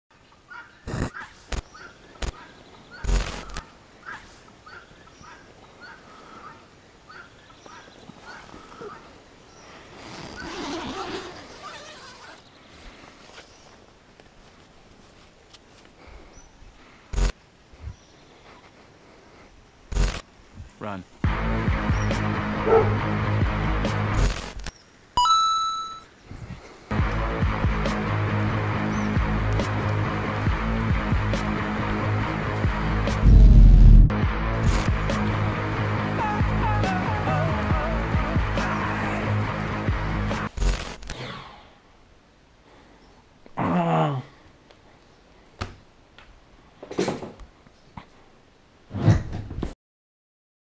Vale dos sonhos
Acordando no acampamento no vale dos sonhos e União-PI.
Começar o dia assim não tem preço, cada um tem seus momentos de felicidades e seus gostos, mas eu prefiro a minha forma de viver.